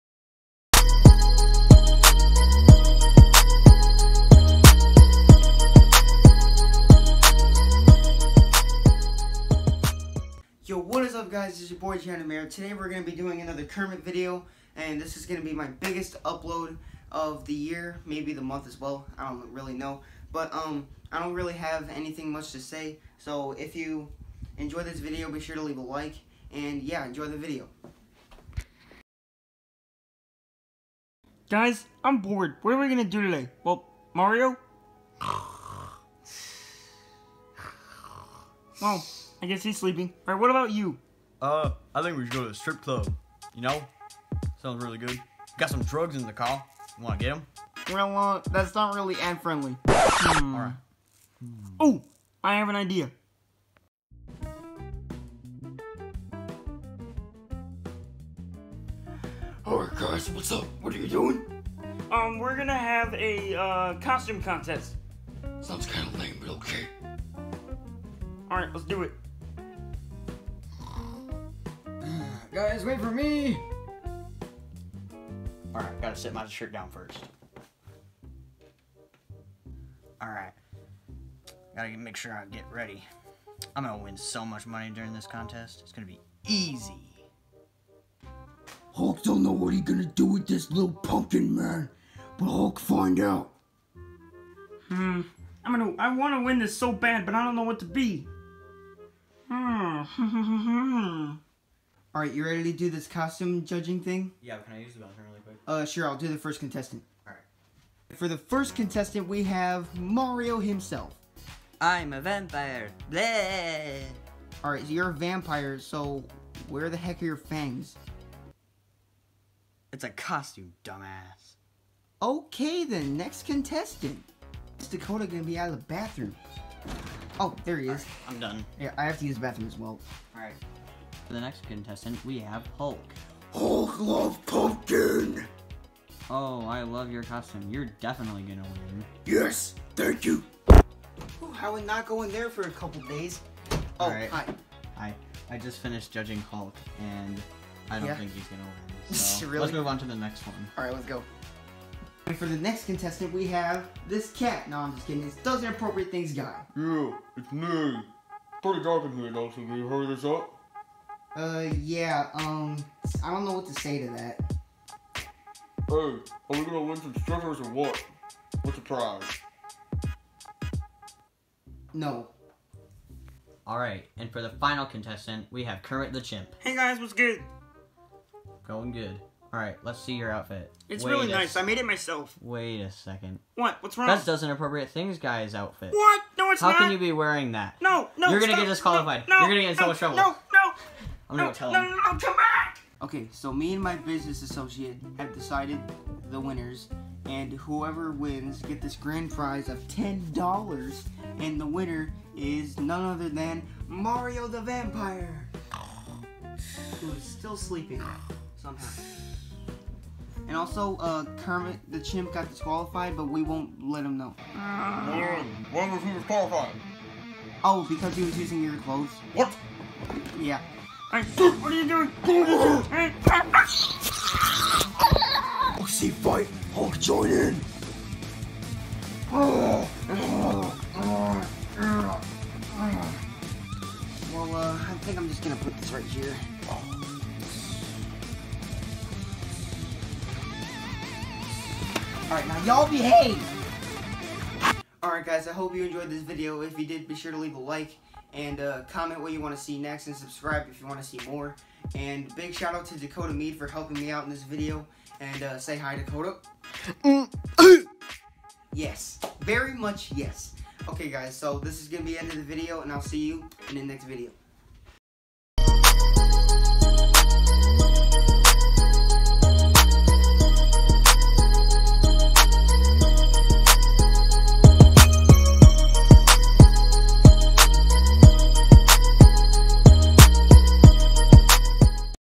SML Parody: Cermit the Chimp's SPOOKY Halloween!!!
This is the first ever SML Parody on my channel! In this episode, Cermit the Chimp, Does Inappropriate Things Guy, and a bunh of other new characters have a Halloween costume contest!